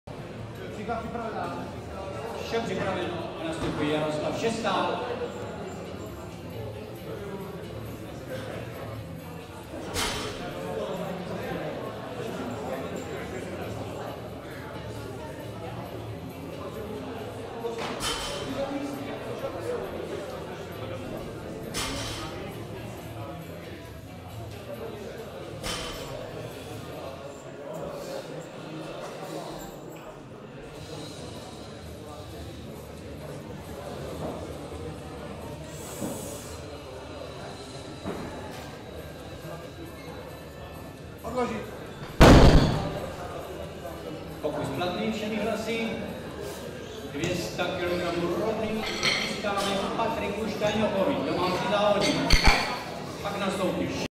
Šesták Jaroslav (2000), MT2, 182,5 kg
1. Mistrovství Moravy v klasickém (RAW) silovém trojboji dorostu a juniorů

Místo: Hornický dům, Zbýšov